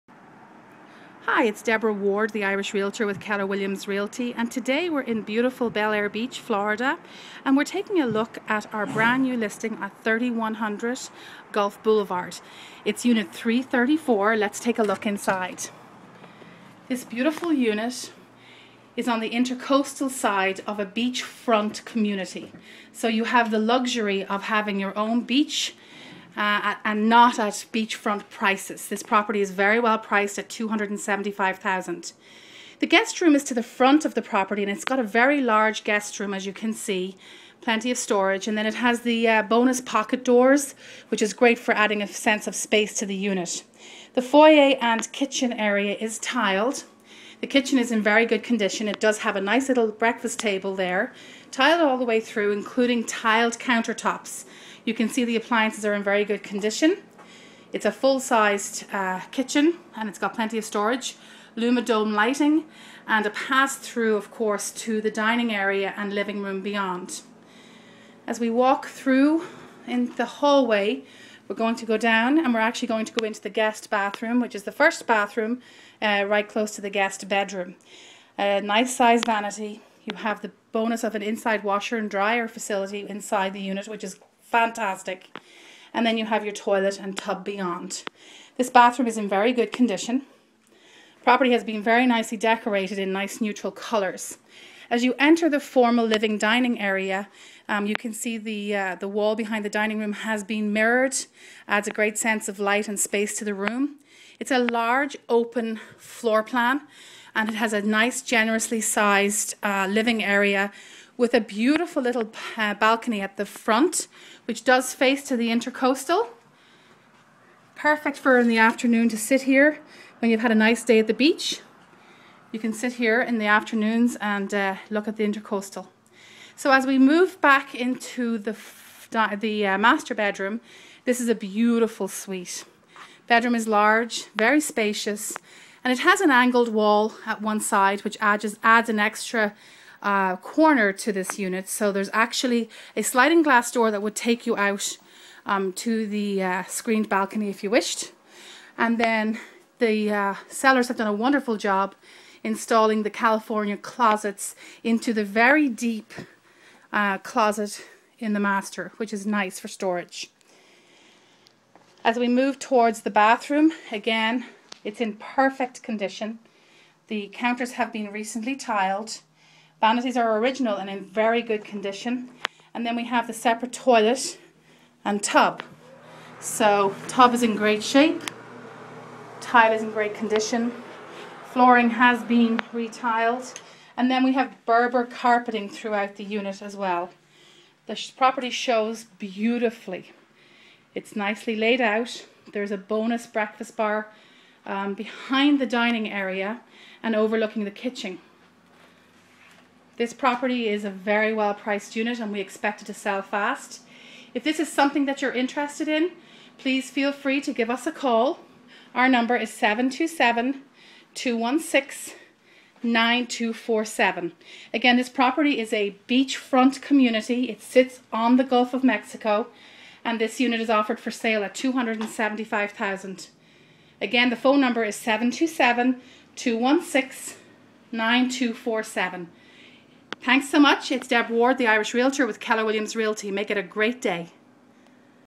Belleair Beach, Florida - Gulf front condo complex - 3100 Gulf Boulevard
- Deborah Ward, Keller Williams Gulfside Realty presents 3100 Gulf Blvd. Enjoy beachfront living without the beachfront price! This spacious unit faces the intercoastal, and while this unit does not have Gulf front views, you'll enjoy all the amenities of this prestigious Gulf front community! This unit offers generously sized bedrooms, large living/dining room combination, breakfast bar, washer and dryer hookups inside the unit and much more! The full-size kitchen features a pass-through to the dining area -- great for entertaining! The private balcony is perfect to enjoy the Clearwater cityscape and intercoastal views. The sparkling community pool, private beach, covered parking and great location make this a rare find in the heart of Belleair Beach!  This two bedroom , two bath unit is priced to sell at $275,000.